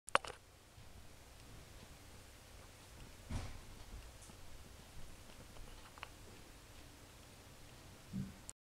Arduino powering another through MIDI connection
An arduino is wired to another using a standard MIDI cable. The second Arduino is powered by the first one and also receives MIDI messages. In this case, the first Arduino sends a Program Change #5 and the second Arduino is programmed to flash its LED upon receiving a Program Change.